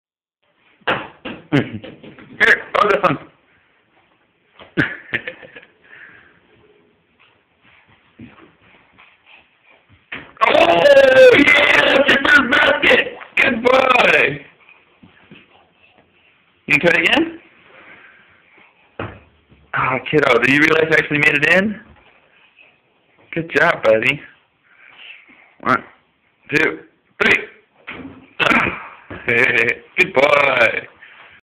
Logan playing basketball
his first ever basket - LA Lakers here we come!